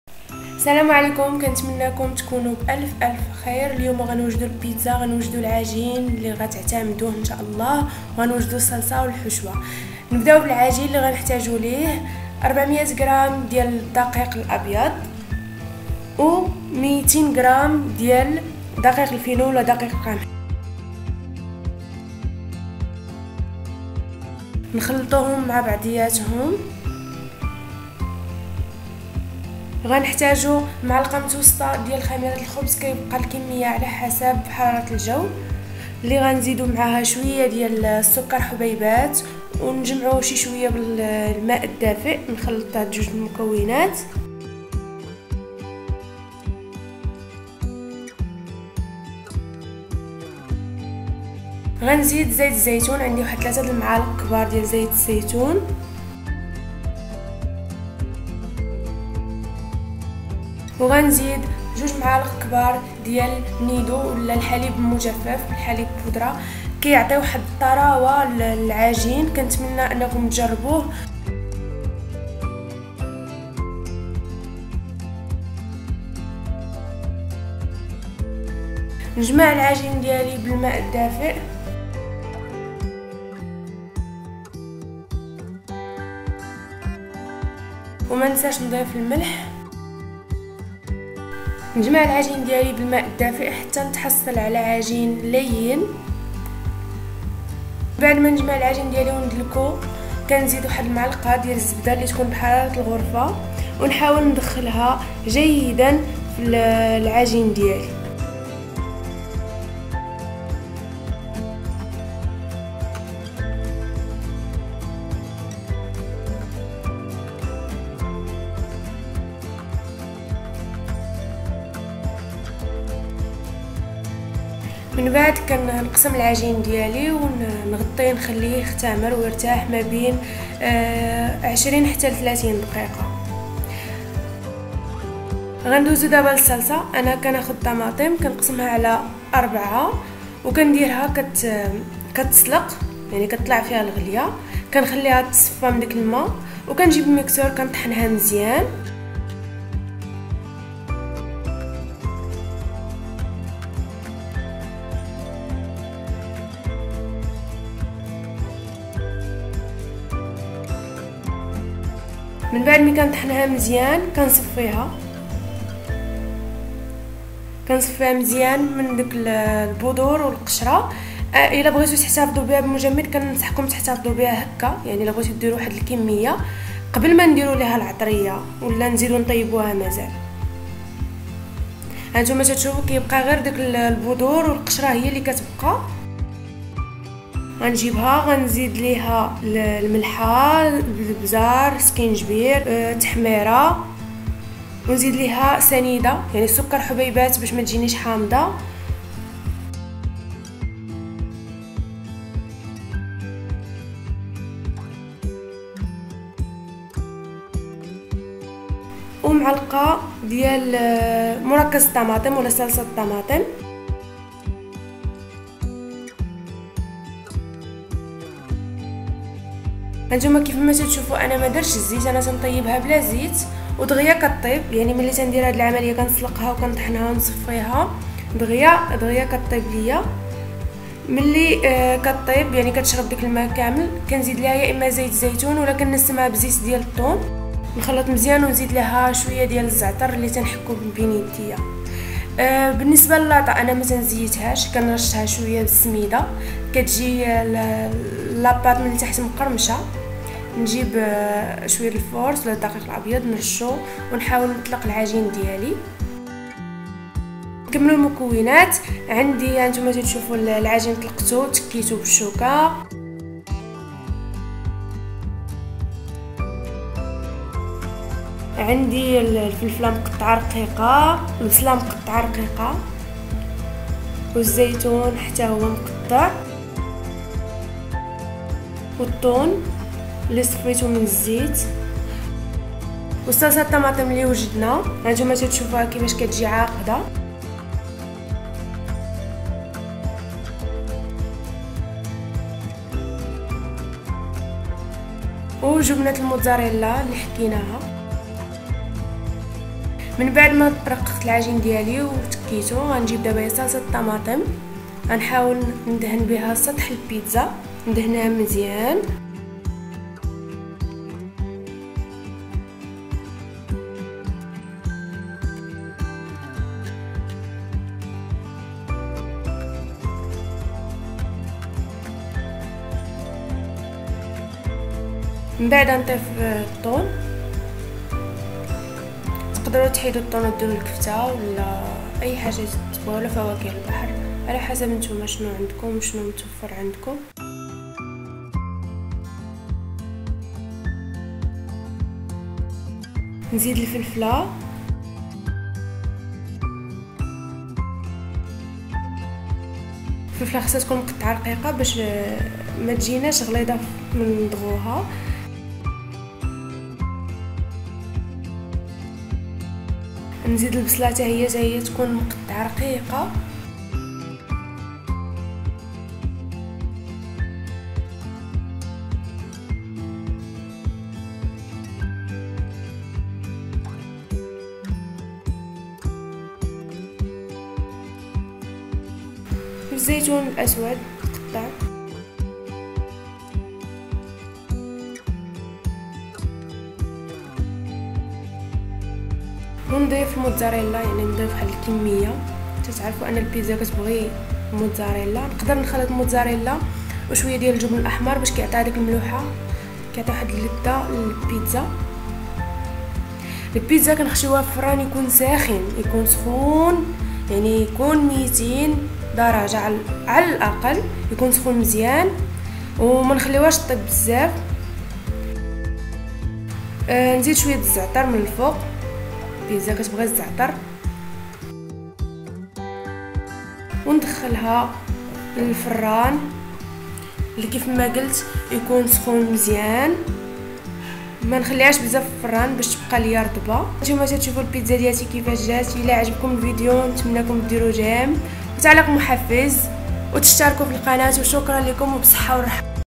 بيتزا هت بعجين رطب ناجح و صلصة مميزة روعة / Pizza Hut#
المقادير
للعجين
400 غ دقيق ابيض
200 غ دقيق القمح
ماعقة متوسطة خميرة الخبز
سكر ملح
ملعقتان حليب مجفف
3 ملاعق زيت الزيتون
ماء دافئ للعجن
صلصة الطماطم
طماطم
ملح ابزار زنجبيل تحميرة سكر
مركز الطماطم
زيت الزيتون
بصل
فلفل
زيتون اسود
جبن موزاريلا
زعتر